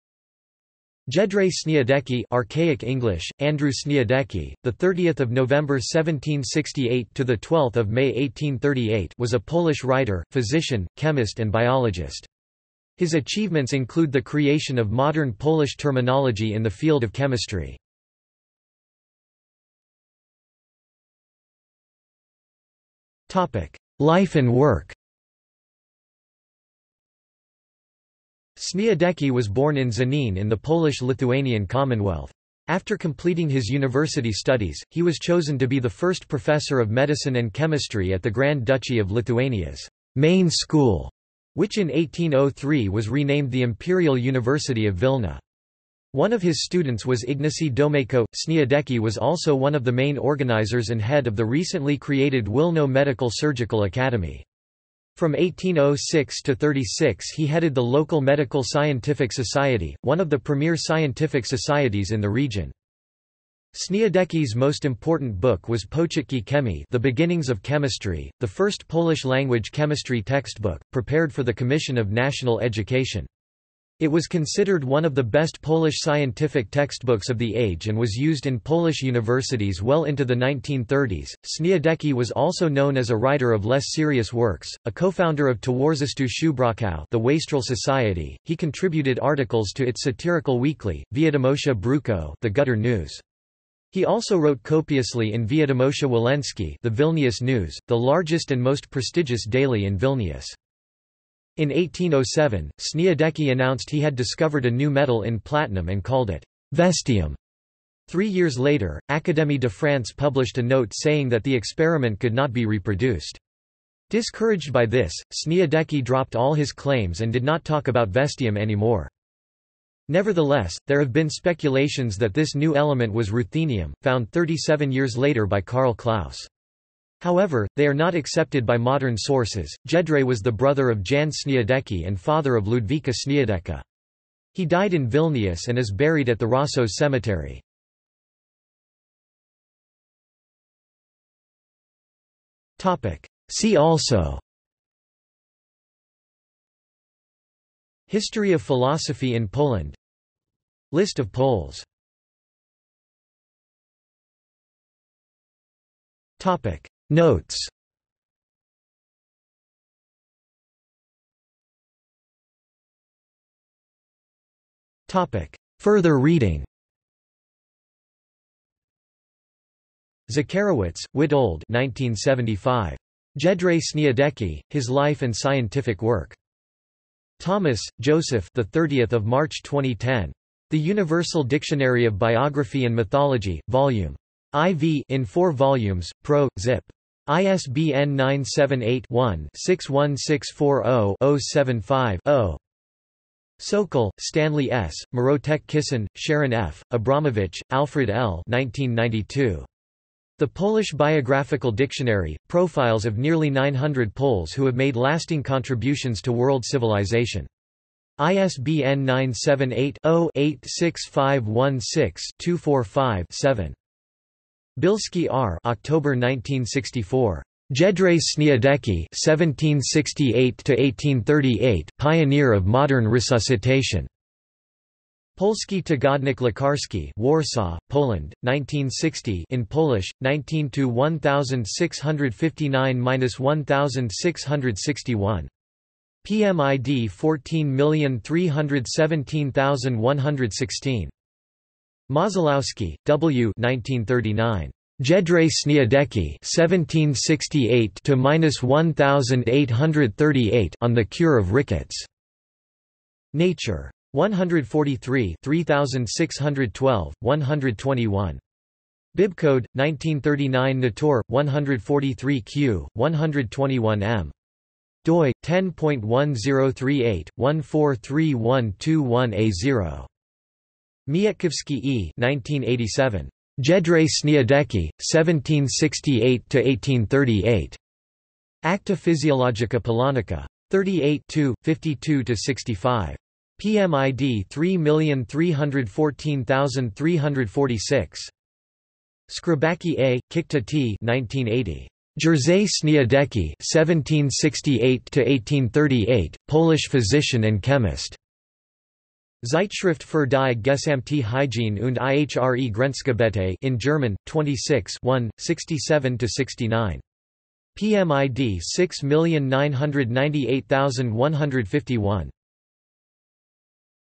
Jędrzej Śniadecki | Wikipedia audio article
This is an audio version of the Wikipedia Article:
Jędrzej Śniadecki

00:00:19 1 Life and work
00:02:19 2 See also
00:02:32 3 Notes
00:02:41 4 Further reading

- Socrates

SUMMARY
Jędrzej Śniadecki (archaic English: Andrew Sniadecki; 30 November 1768 – 12 May 1838) was a Polish writer, physician, chemist and biologist. His achievements include the creation of modern Polish terminology in the field of chemistry.